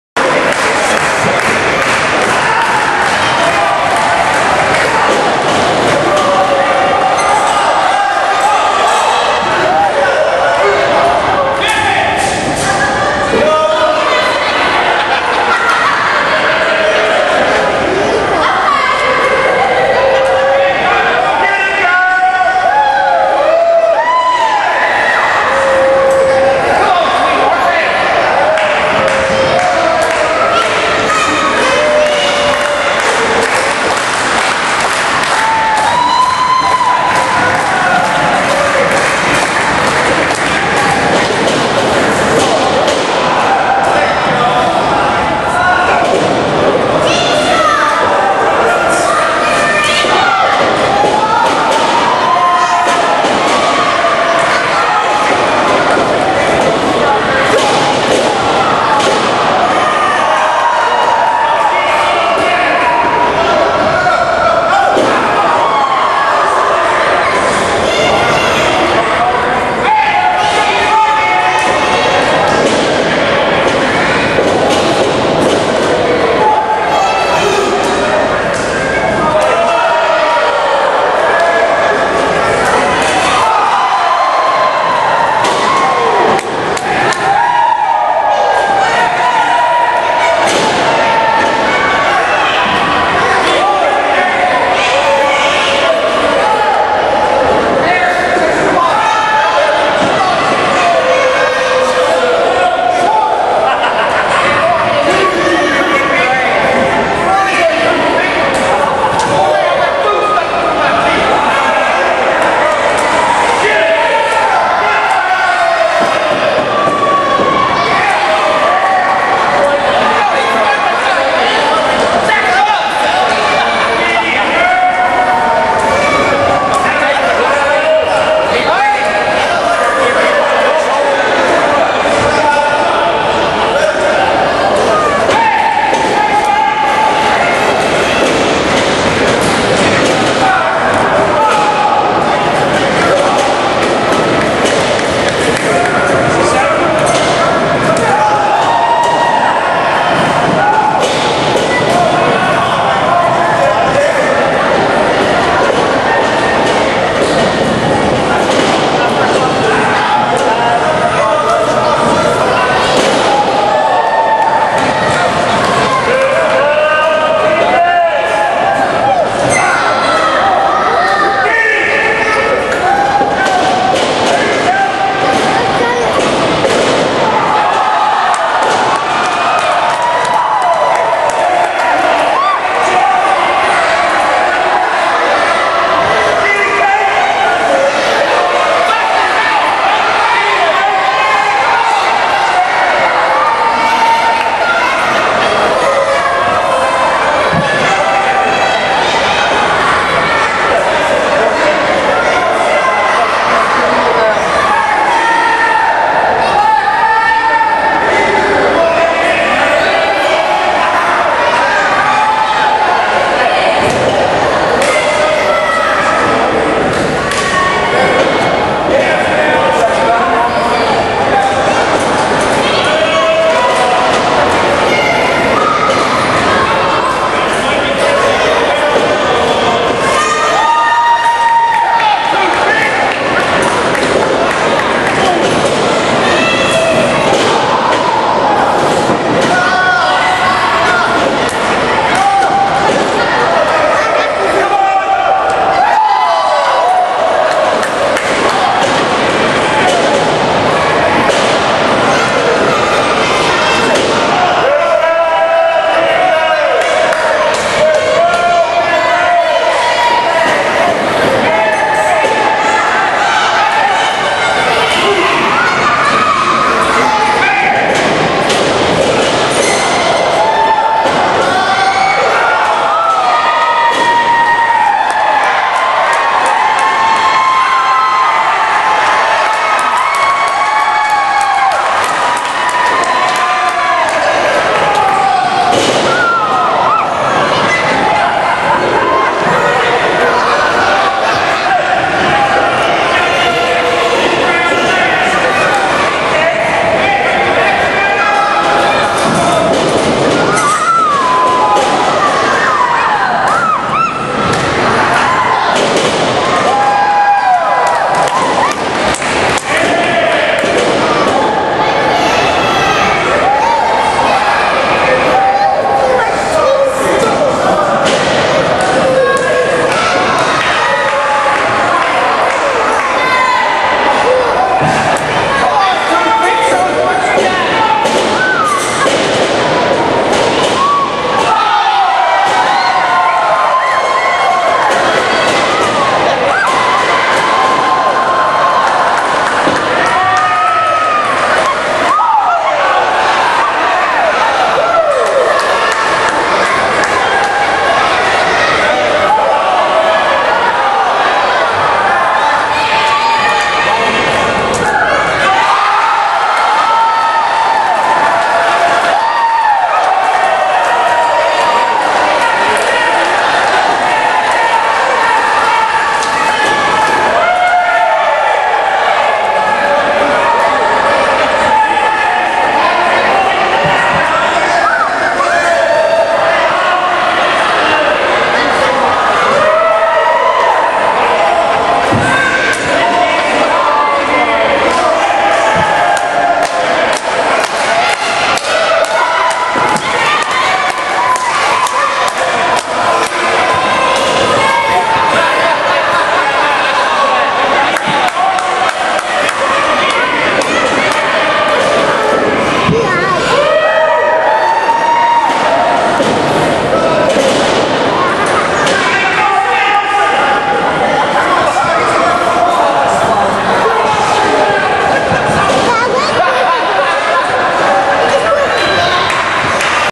Usual Suspects vs. Team G.A.Y. - Part 1
Part one of The Usual Suspects with McLovin vs. Team G.A.Y.  at an XMCW event filmed on March 28, 2009.